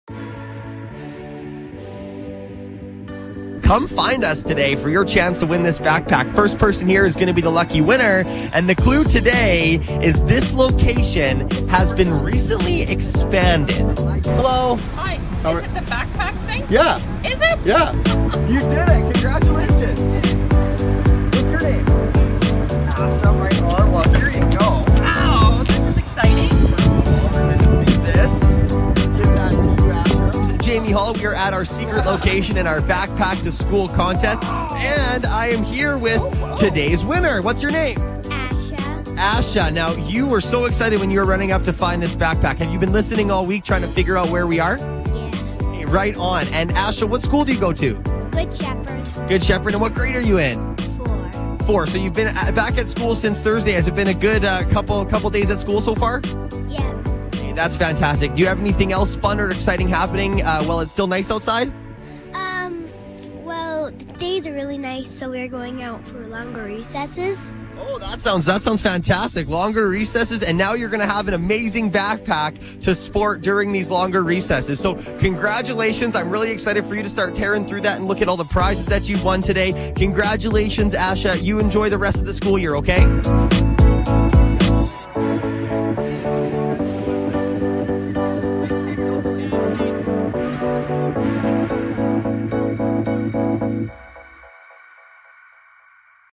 Backpack To School 7
Today our lucky winners for the sixth day of Back Pack to School were Jakki and Nina! Congratulations!